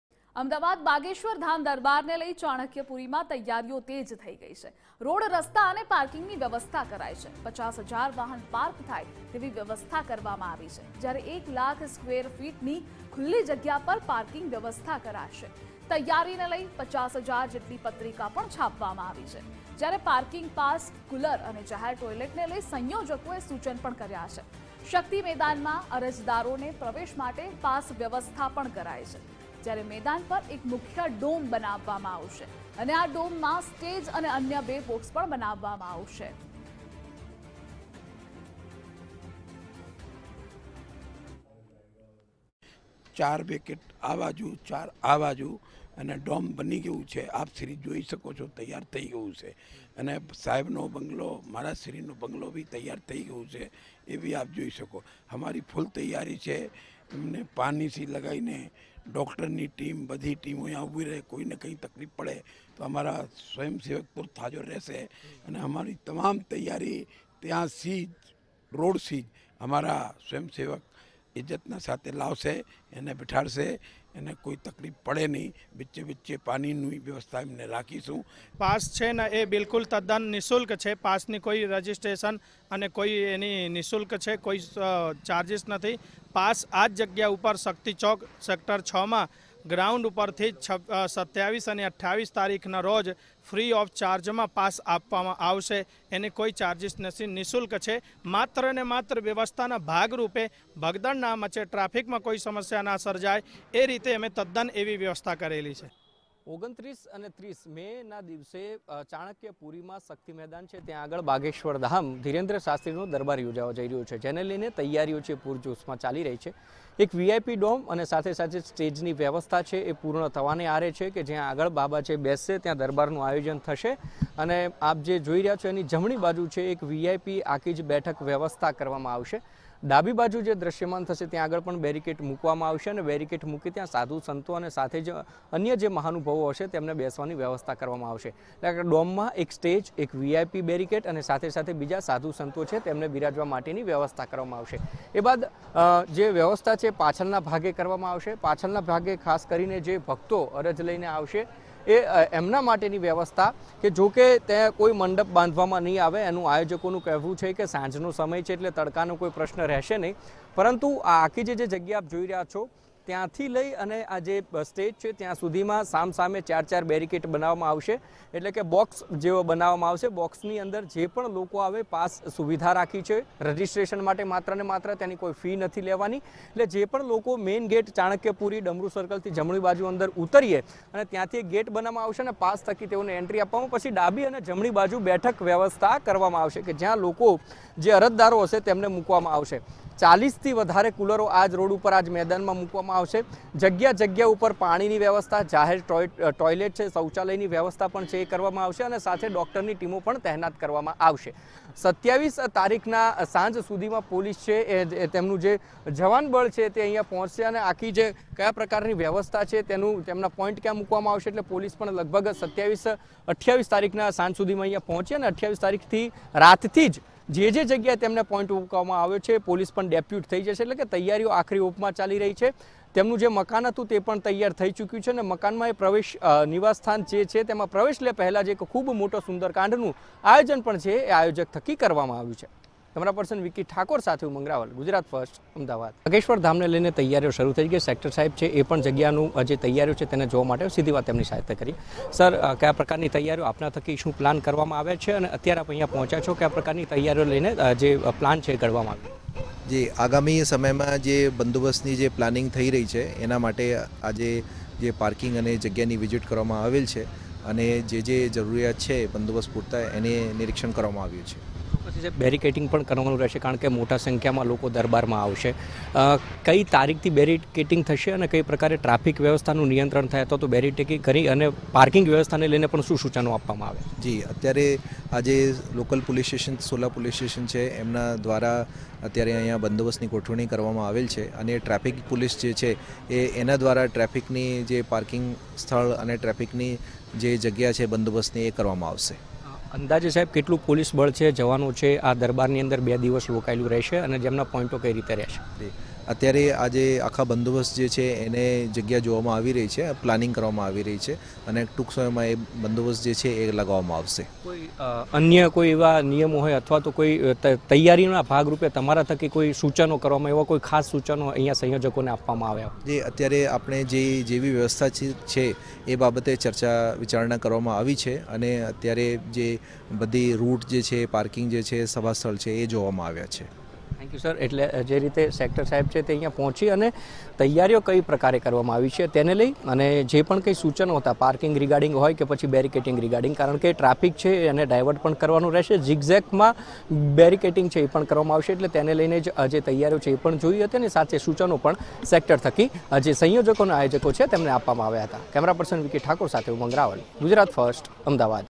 Ahmedabad : Bageshwar Dham દરબારને લઈ ચાણક્યપુરીમાં તૈયારીઓ | Dhirendra Shastri | Gujarat First
બાગેશ્વર ધામ દરબારને લઈ ચાણક્યપુરીમાં તૈયારીઓ
રોડ રસ્તા અને પાર્કિંગની વ્યવસ્થા કરાઈ
50 હજાર વાહન પાર્ક થઈ શકે તેવી વ્યવસ્થા
1 લાખ સ્કેવરફીટની ખુલ્લી જગ્યા પર પાર્કિંગ વ્યવસ્થા કરાશે
તૈયારીને લઈ 50 હજાર જેટલી પત્રીકા પણ છાપવામાં આવી
પાર્કિંગ પાસ કુલર અને જાહેર ટોઈલેટને લઈ સંયોજકોને કર્યુ સૂચન
શક્તિ મેદાનમા અરજદારોને પ્રવેશ માટે પાસ વ્યવસ્થા પણ કરાઈ
મેદાન પર એક મુખ્ય ડોમ બનાવામા આવશે
ડોમમા સ્ટેજ અને અન્ય બે બોક્સ બનાવાશે